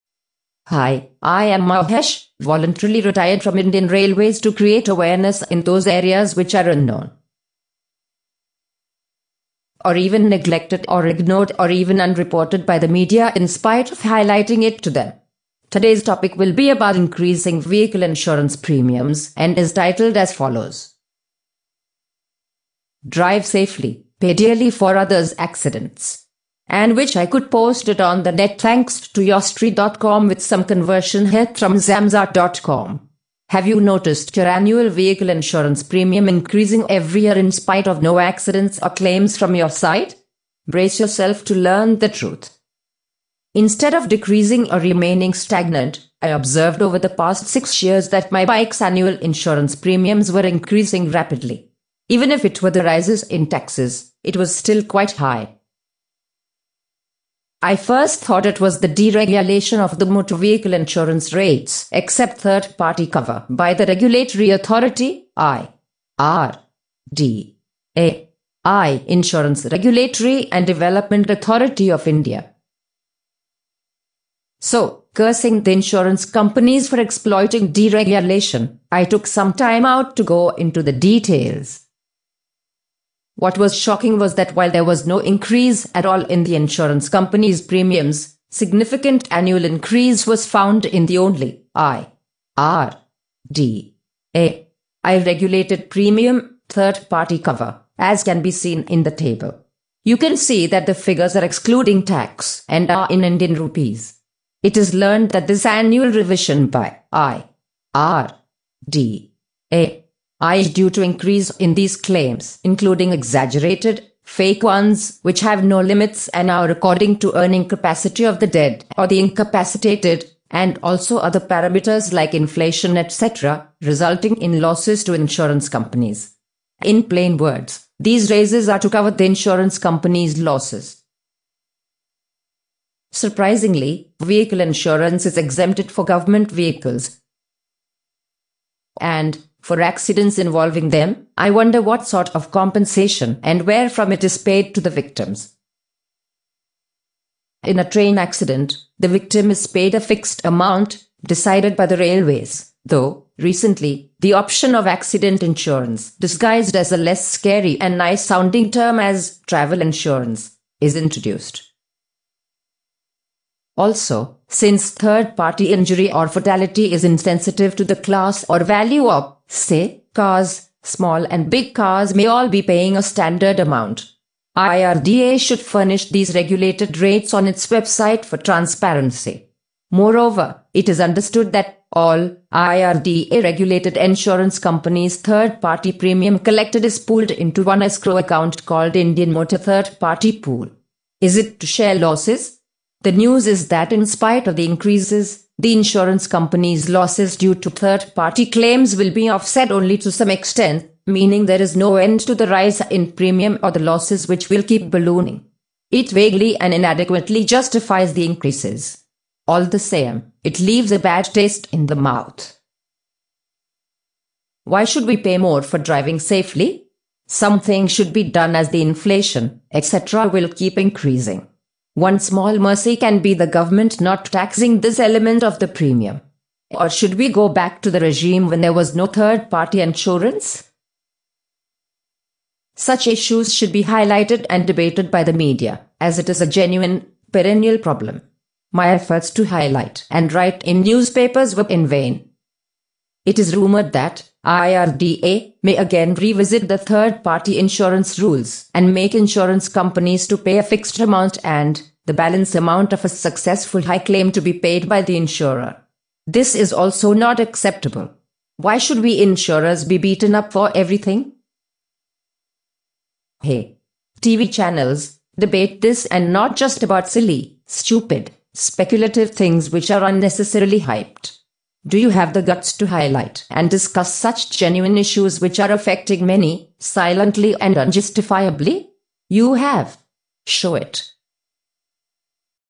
DRIVE SAFELY, PAY DEARLY FOR OTHERS' ACCIDENTS
ABOUT UNJUSTIFIED ANNUAL INCREASE IN MOTOR VEHICLE INSURANCE PREMIUMS IN INDIA